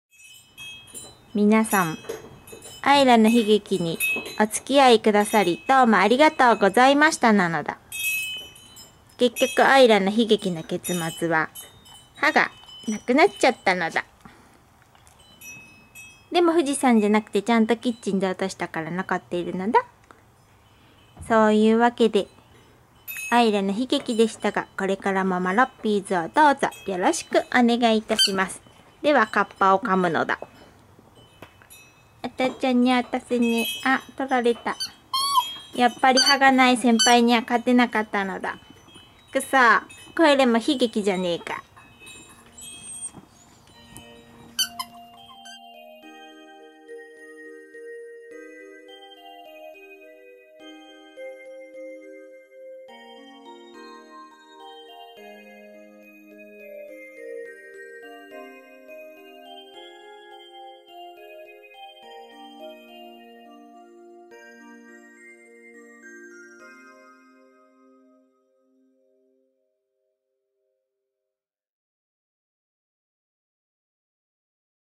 ㌧の悲劇「完結編」
長らくおつきあいくださいましてありがとうございました。

結局、この騒動のさなかに立て続けに㌧とチカの歯が抜けて悲劇は完結いたしました。

Marroppy's！